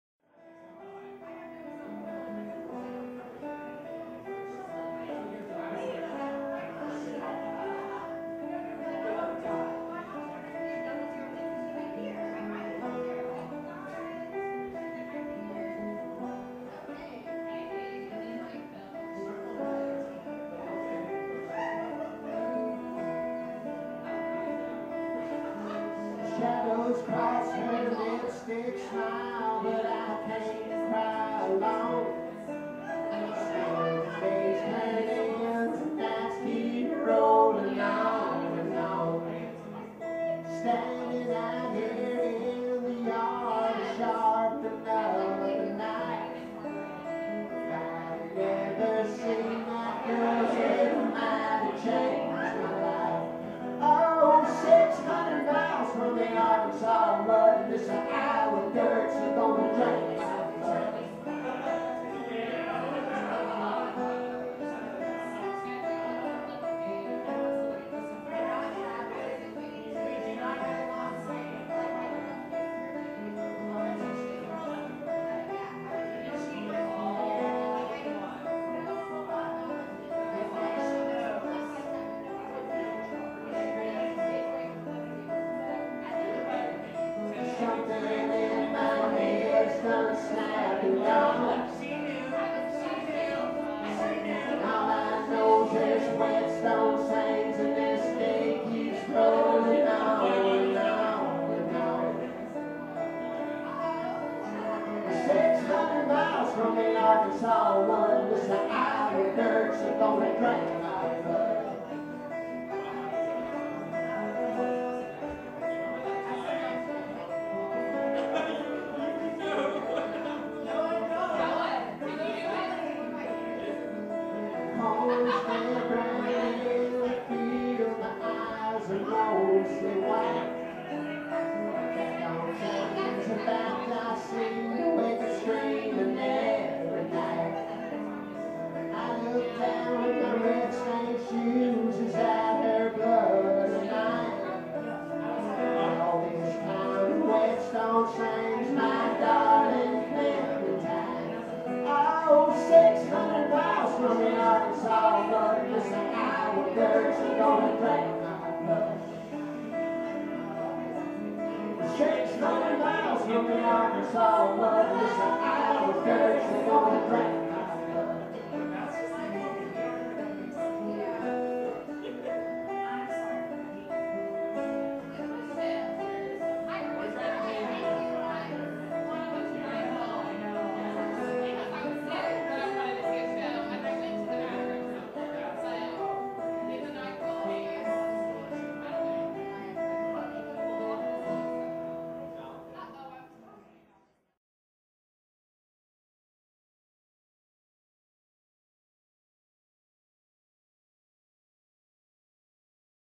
The Victor Mourning @the birdhouse
The Victor Mourning perform acoustic anachronisms from another century that are firmly routed in this one—sometimes raucous, sometimes quiet, mostly dark, and very southern. Thought provoking, starkly haunting songs tell tales of grief torn outcasts, confidences betrayed, and death by murder. There's a song about a man who eats nothing but locusts and Campbell's soup, another about a traveling albino, and an unexpected version of the ancient Greek myth of Icarus set in the hills of rural Arkansas. With classic storytelling, soaring harmonies, and melodic fiddle, the band creates music with an old time feel filtered through a modern sensibility, at once both relevant and true to its ancient roots.